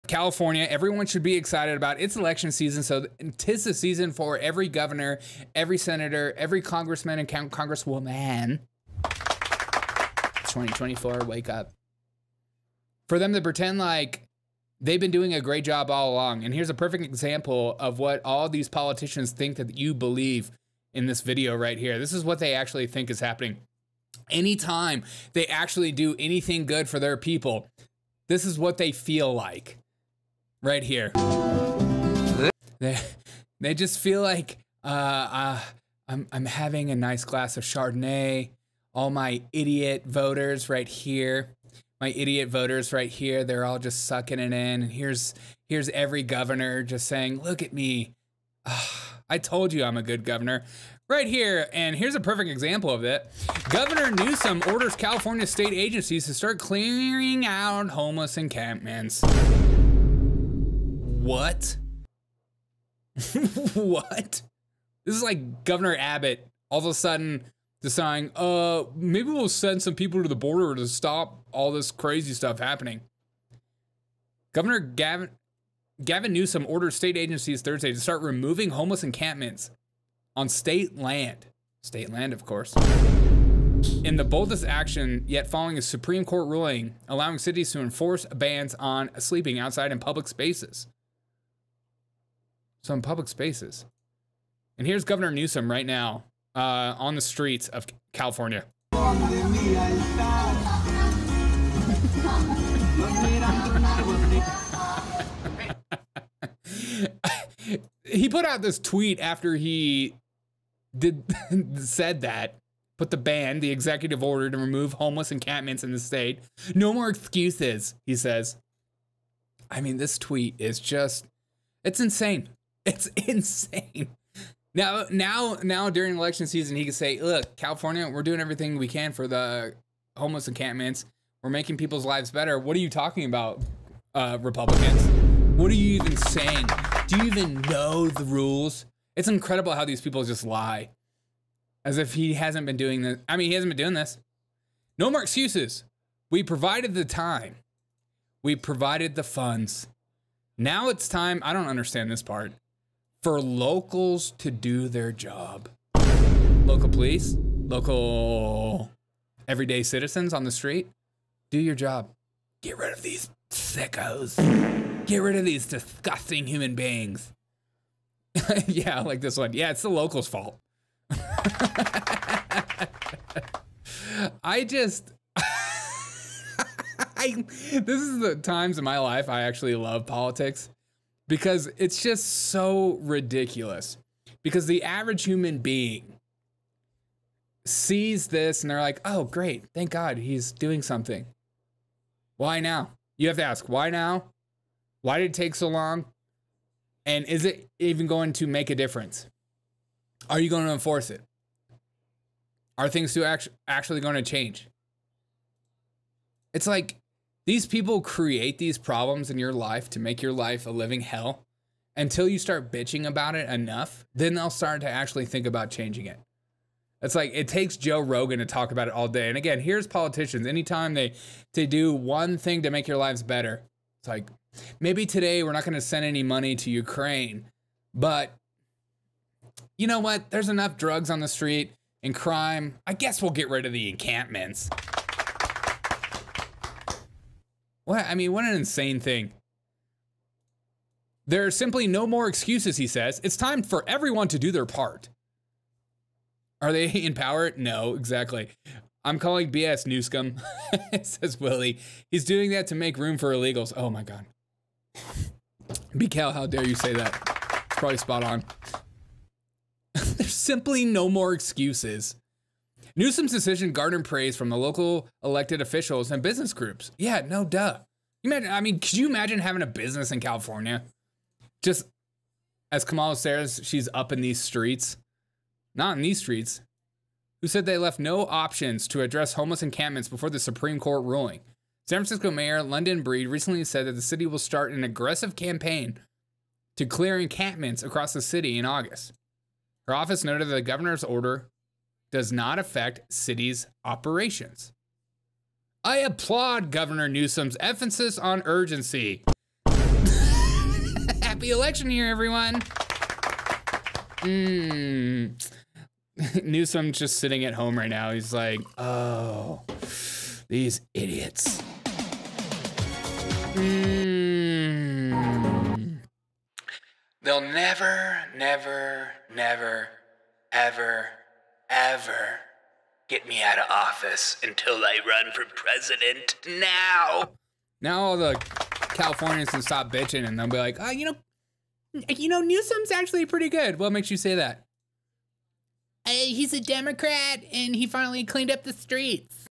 Gavin Newsom's Deceit about Working for Californians
Newsom orders homeless camps cleared following Supreme Court ruling